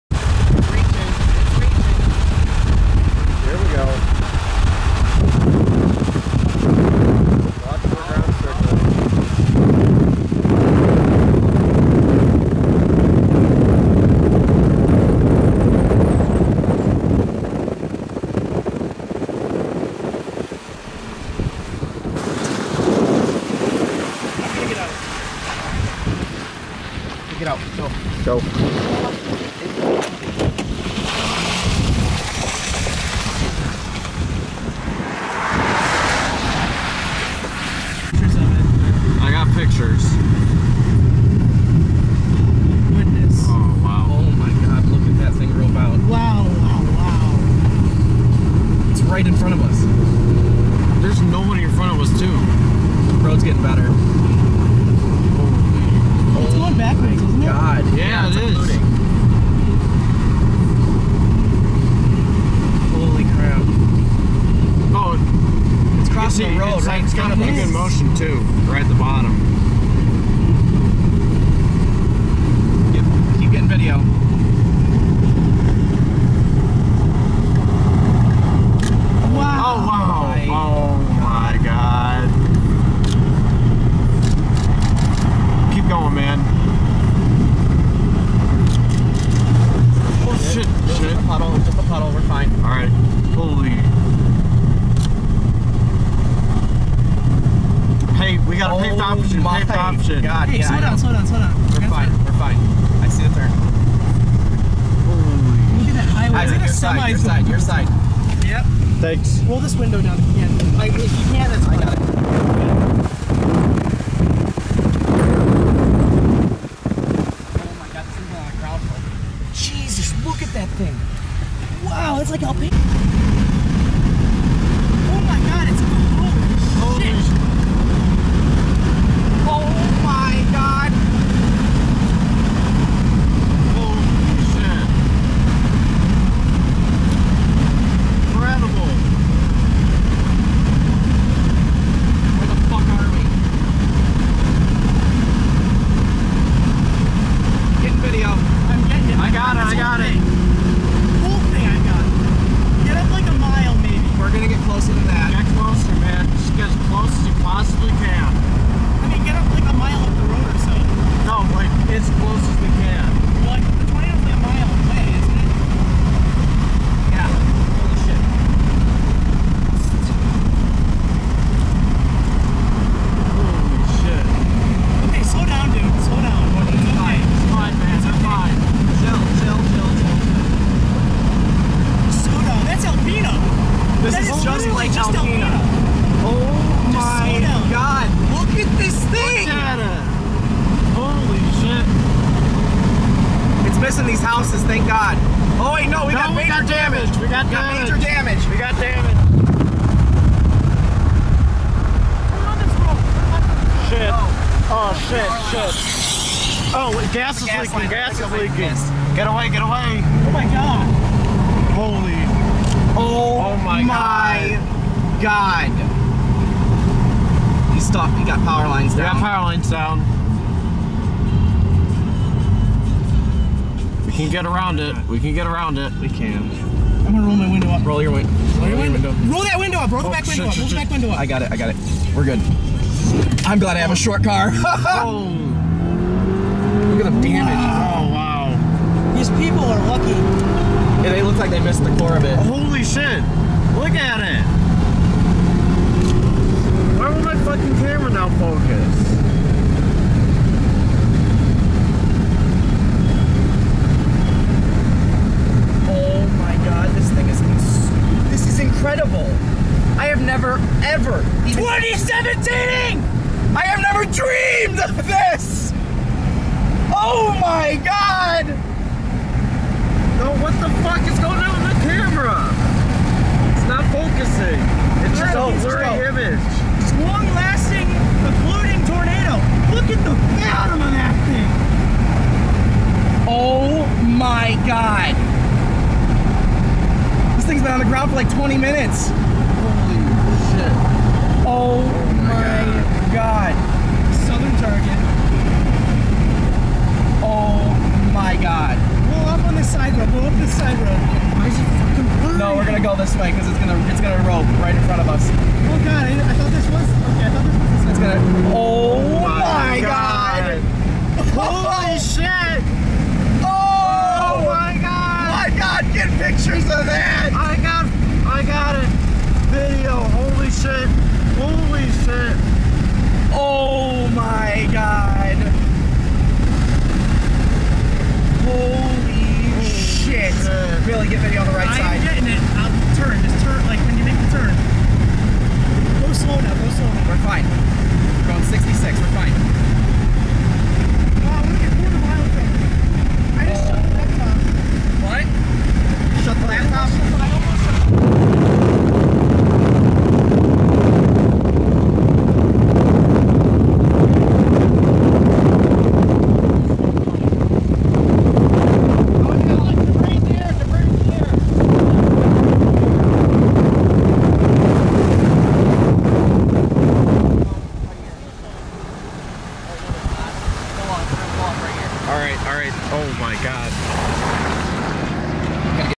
Carpenter Tornado June 12th, 2017 Full Video
EF2 tornado went through parts of N Colorado and SE Wyoming damaging several structures along with trees and power poles. It was intense, hence the swearing.